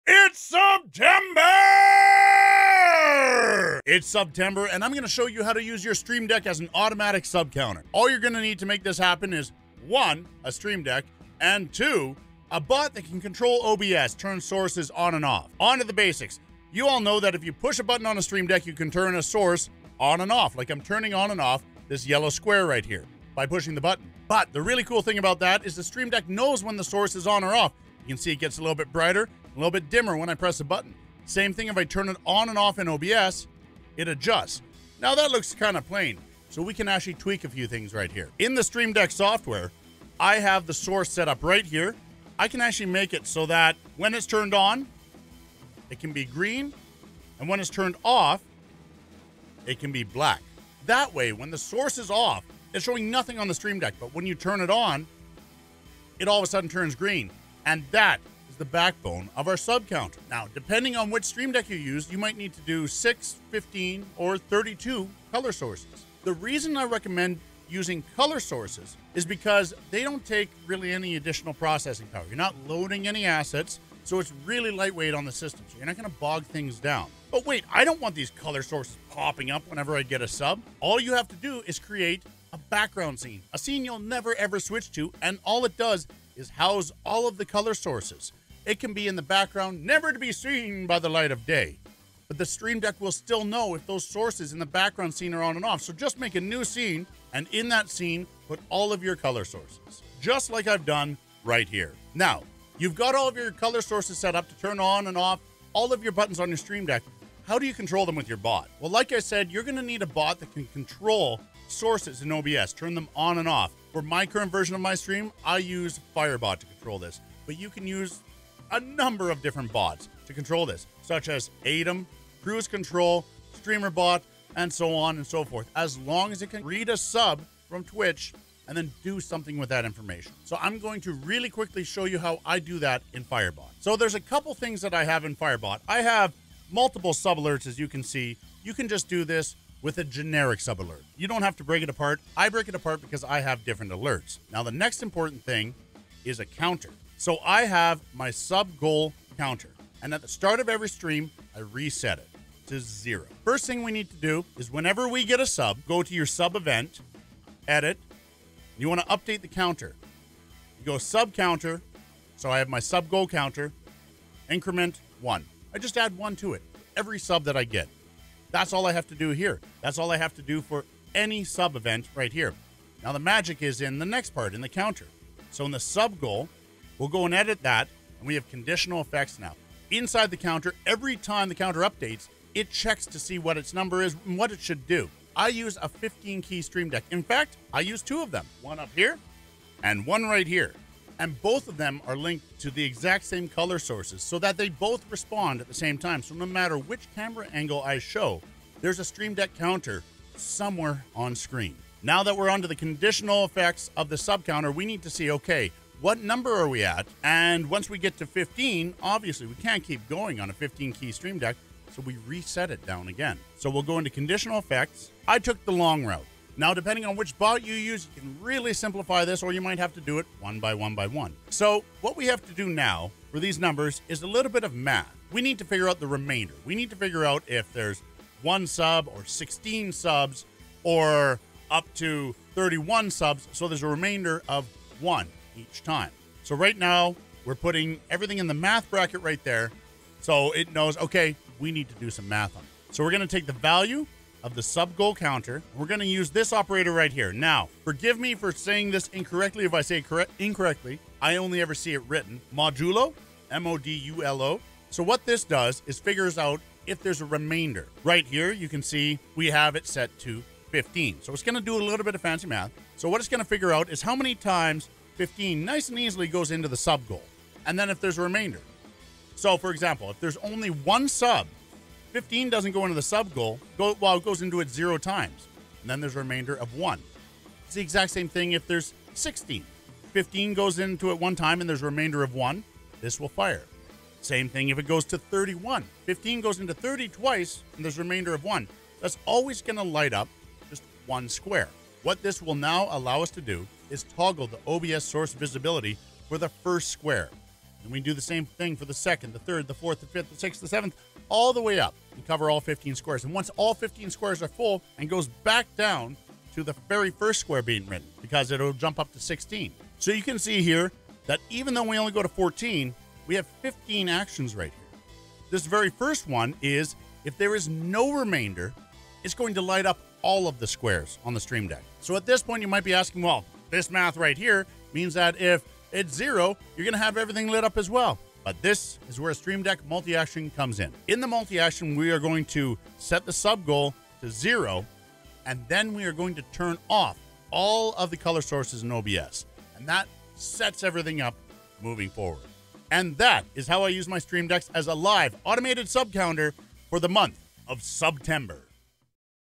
Automatic SUBtember Stream Deck Sub Counter
I use 2 of my Elgato Stream Decks as live, automated sub counters that display my current sub count in my studio from all camera angles, and I'm showing you how I did it.

My unique twist on this is that every time the 15 key Stream Deck is filled by subs, I take a shot of maple syrup.